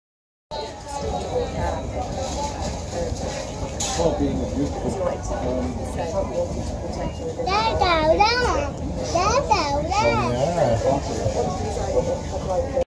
Amazing Dancing Seagull
What is this seagull doing? Dancing like his feet are on fire!